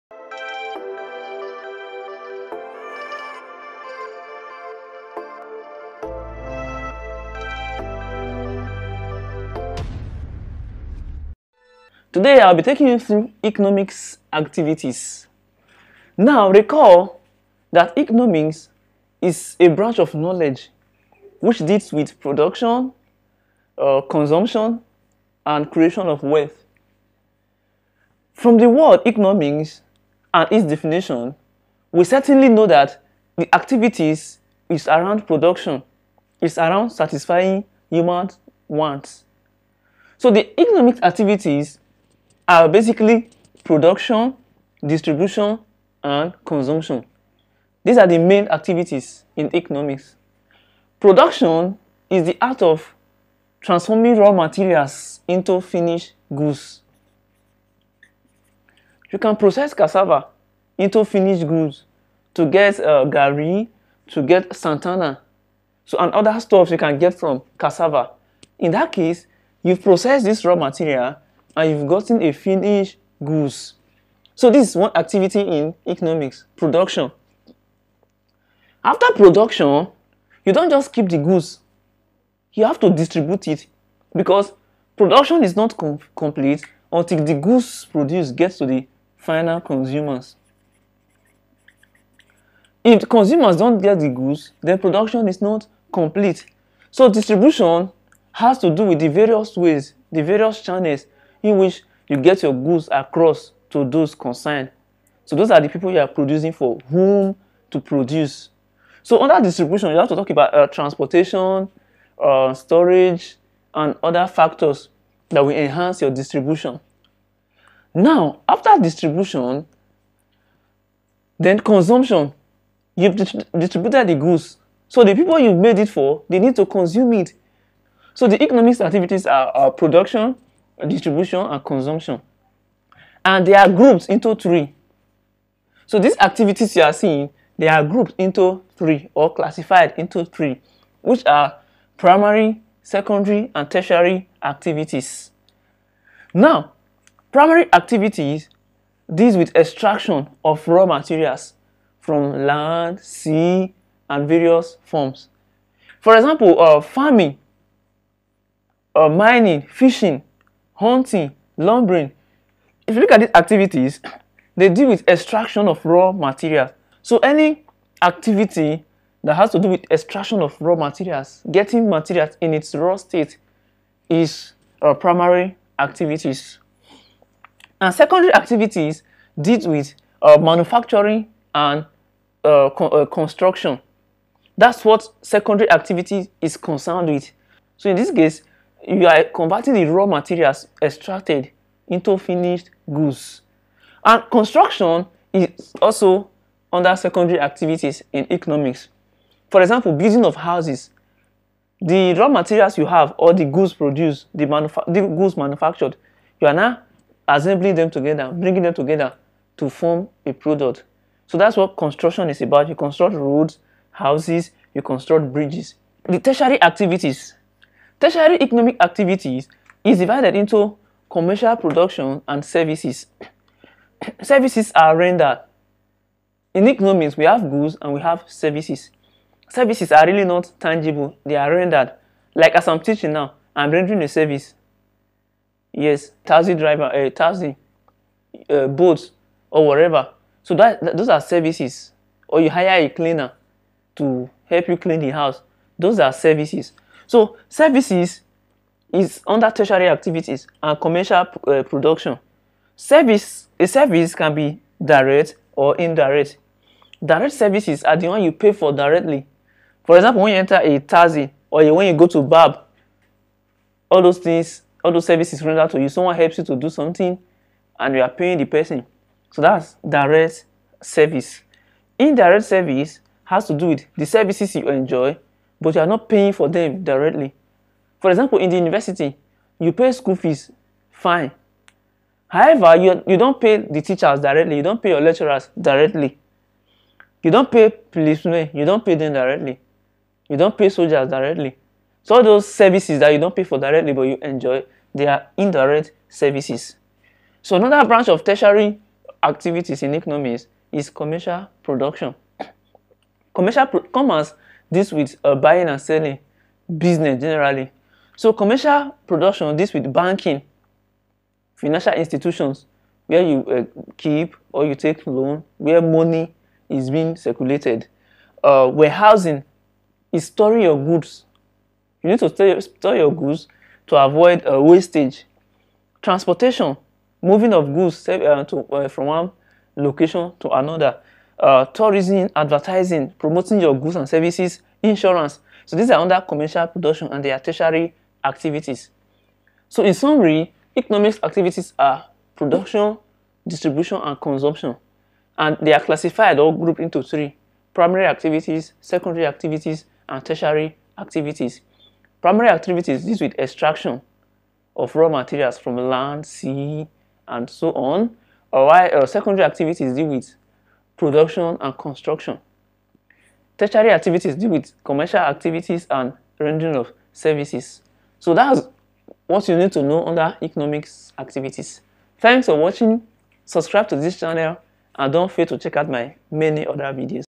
Economics Activities - Meaning And Classes
Economics activities like Production, Distribution and Consumption. Classification of Economics Activities into Primary, Secondary and Tertiary Activities...

With this Video, you should be able to answer as many questions as possible in Waec, Jamb And Post UTME under the classes of Economics.